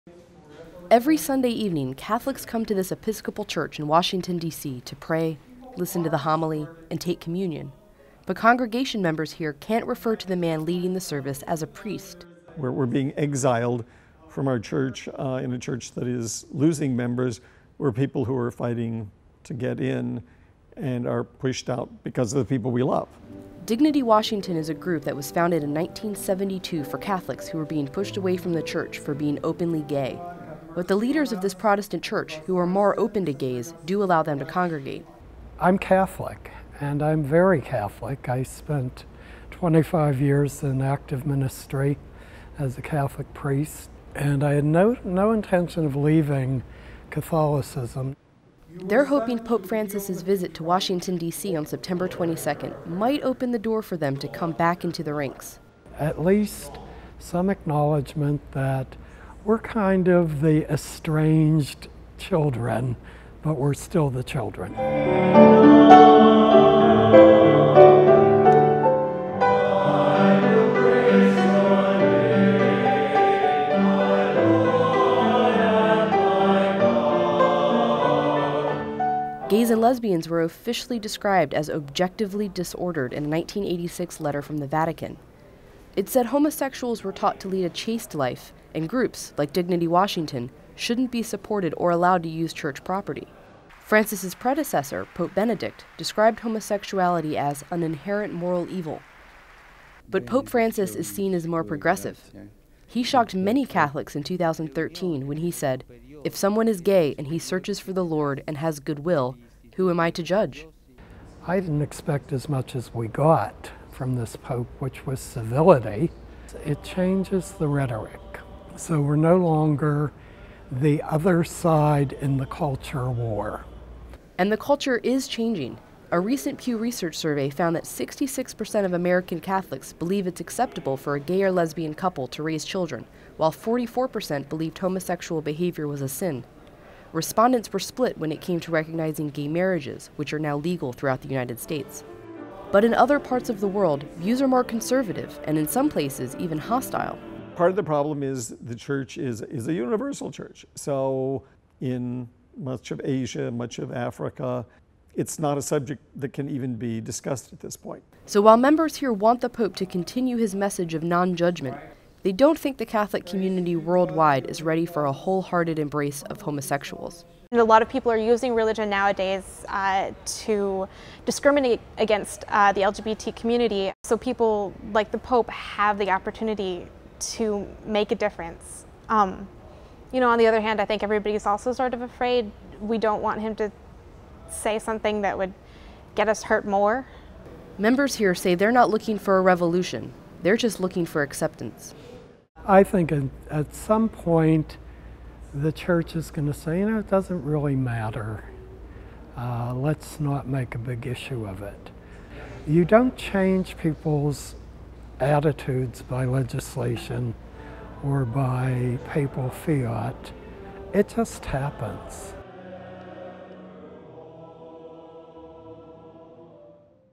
Gay Catholics Seeking Church Acceptance Pin Hopes on Pope
Dignity Washington, a group of LBGT Catholics in Washington, D.C., are excited about Pope Francis’ visit on September 22nd and are hoping his message of non-judgement continues as they seek acceptance from the church.